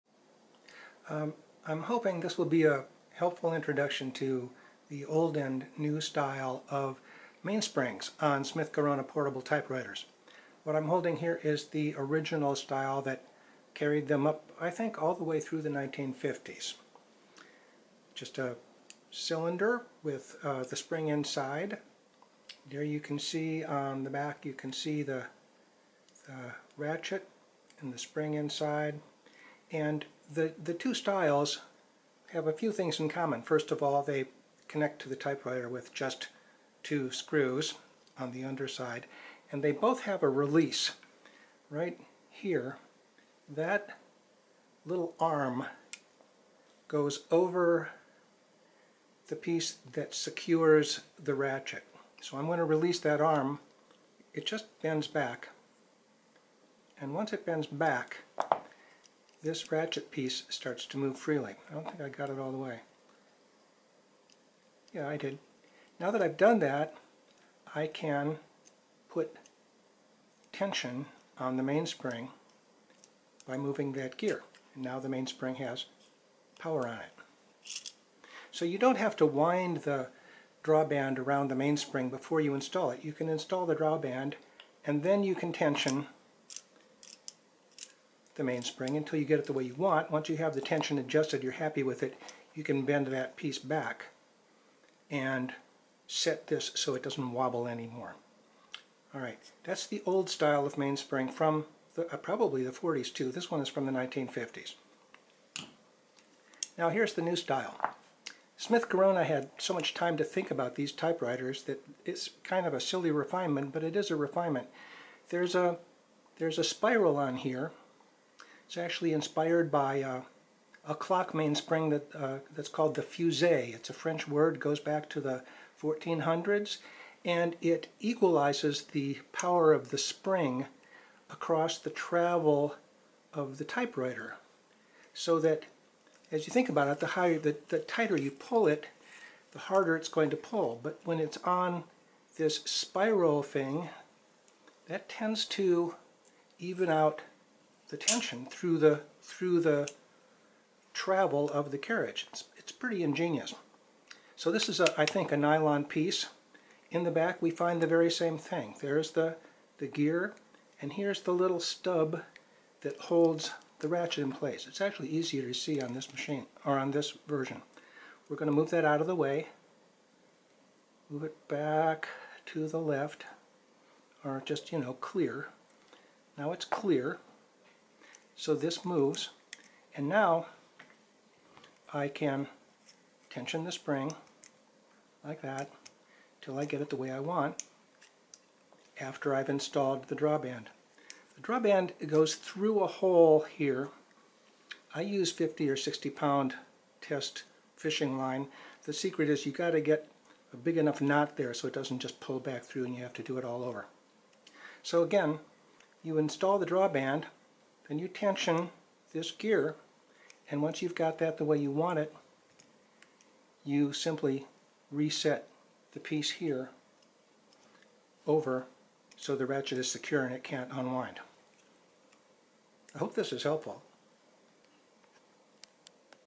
Smith-Corona typewriter mainspring intro
Here's a quick look at the two styles of mainspring used by Smith-Corona -- the first carried through the 1950s, and the second was introduced sometime in the early 1960s. The principles are the same, but the newer unit uses a creative approach that helps keep the spring's "pull" more constant through the travel of the carriage.